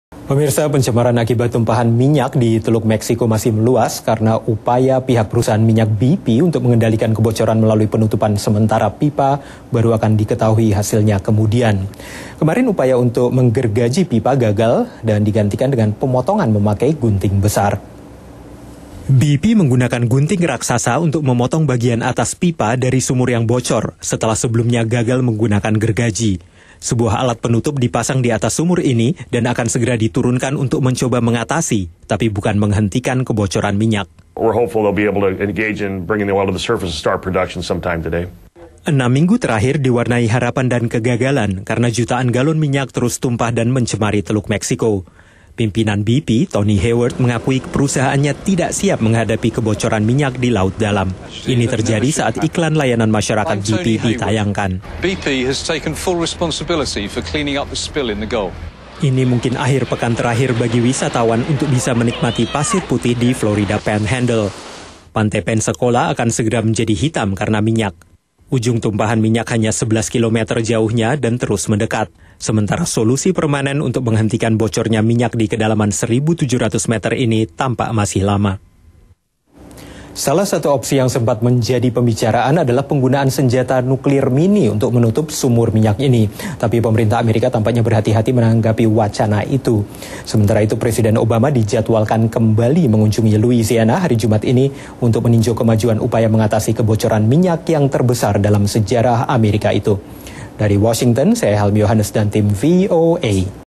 BP Potong Pipa dan Tutup Sementara untuk Atasi Kebocoran - Laporan VOA 3 Juni
Pemerintah AS terus mengawasi upaya BP mengatasi kebocoran minyak di lepas pantai negara bagian Lousiana, sementara tumpahan minyak sudah mendekati pantai wisata di negara bagian Florida.